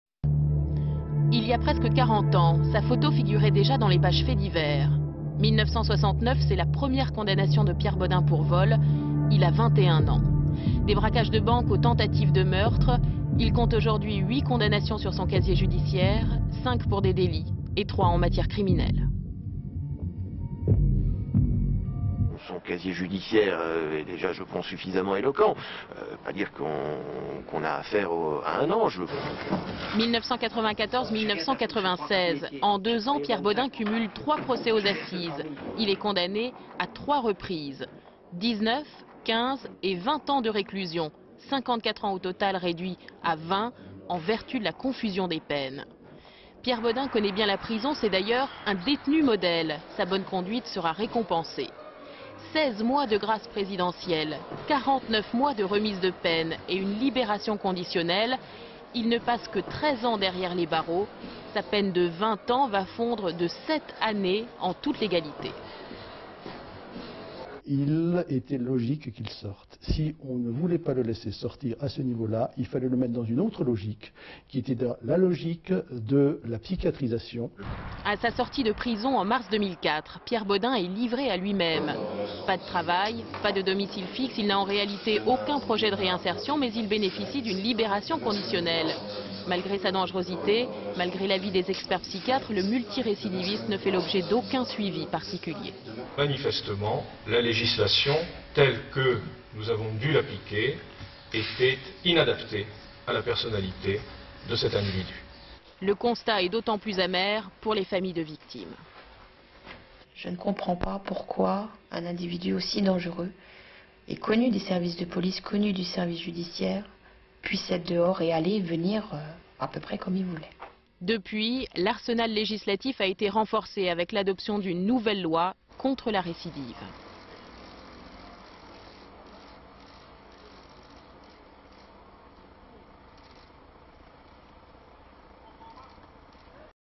Procès Pierre Bodein : portrait du prisonnier libéré
10 avril 2007
Reportage consacré au tueur en série présumé Pierre Bodein, dont le procès s'ouvre demain pour les meurtres d'une fillette et de deux jeunes femmes. Retour sur le portrait de l'homme récidiviste qui avait alors bénéficié de remises de peine et d'une libération conditionnelle.Commentaire sur images d'archives, factuelles, articles de presse et photographies en alternance avec les interviews des anciens avocats de Pierre Bodein, Olivier CHARLES et Laurent HINCKER, ainsi que de Bernard LEGRAS, procureur général de Colmar, et de Françoise SCHARSCH, mère de la victime Julie Scharsch.[Source : prompteur France 2]     Ce qui retient l'attention dans cette affaire, on l'a compris, c'est que Pierre Bodein a bénéficié d'importantes remises de peine... puis d'une libération conditionnelle, sans réelle surveillance...     C'est à ce moment là qu'il a commis ces crimes...     Alors comment et pourquoi a-t-il retrouvé la liberté?...     Enquête, Anouk Burel, Emmanuel Morel Images d'archive INA